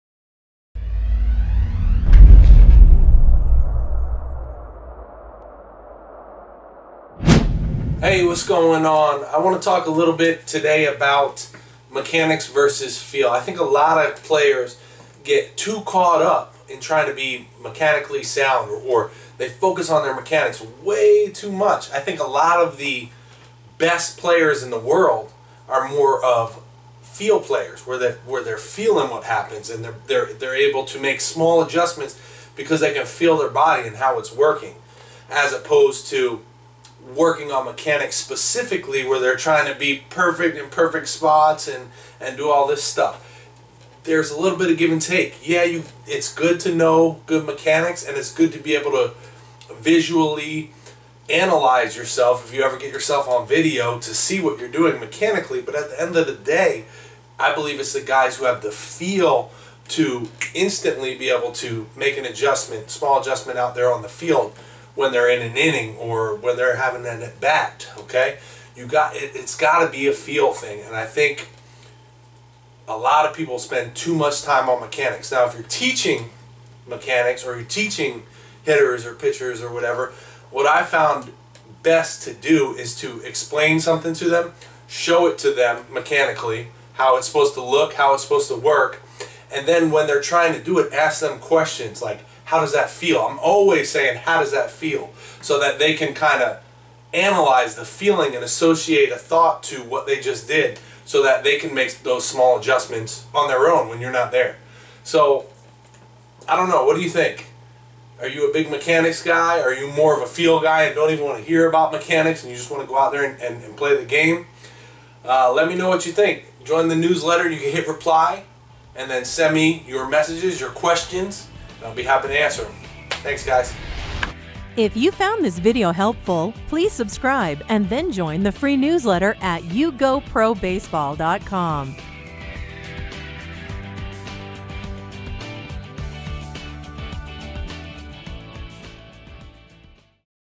Baseball Mechanics vs Feel
Here's a short video where I talk about baseball mechanics vs feel.  I think a lot of baseball players get too caught up on worrying about mechanics.  Now, don't get me wrong, I believe mechanics in pitching, hitting, catching, and fielding are important, but I believe the best baseball players in the game are feel guys.  Guys who can feel when something is wrong or something is right and make small adjustments accordingly.  Baseball is a game of small adjustments and when you can make adjustments quickly, on the spot, in the games, that's when you become a great player.  So that's my take on mechanics vs feel.  So, whether you're a pitcher, hitter, catcher, or whatever, take a little more time to focus on how things feel rather than trying to be perfect with your mechanics.